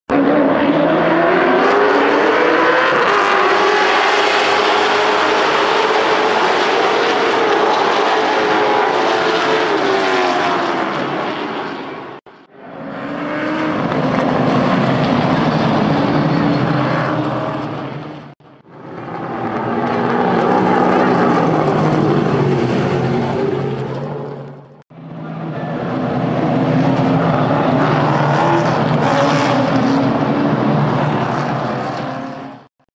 Thanksgiving Classic: Lee Pulliam Vs. Matt McCall
Late race restart that had everybody on their toes. Matt McCall and Lee Pulliam made contact going into turn one during the Thanksgiving Classic at Southern National Motorsports Park. Lee Pulliam ended up finishing 20th while McCall miraculously landed a podium finish.
Apologies for the poor angle. Had we expected something like this happening, we'd have had a camera positioned in that part of the track.